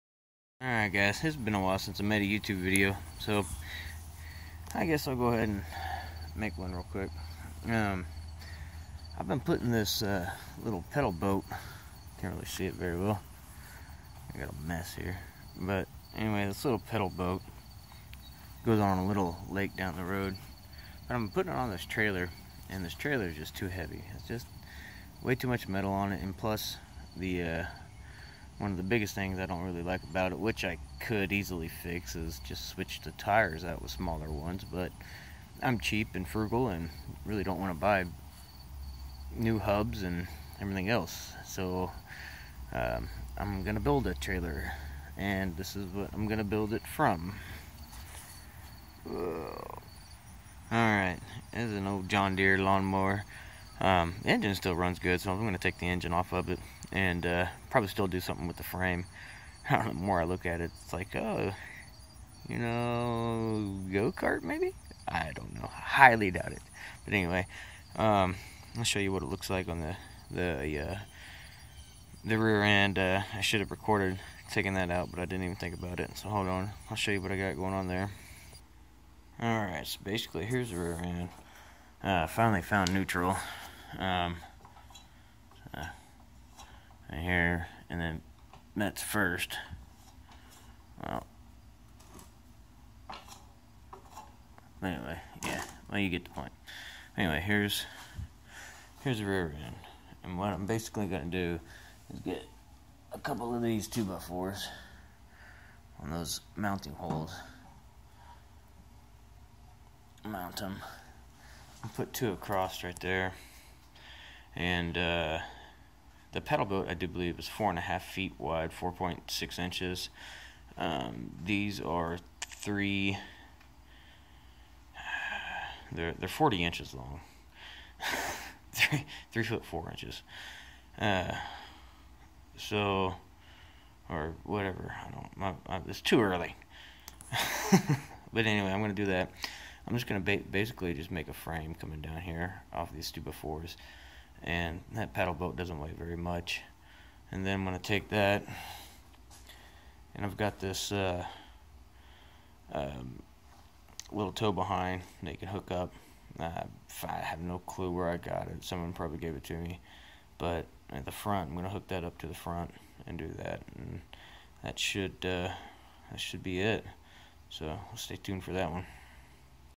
Homemade paddleboat trailer #1
I needed a smaller and lighter trailer for the little paddle boat. Instead of modifying the one I have, I've decided to make a smaller and shorter one. Really not sure if it will work but we're going to try lol!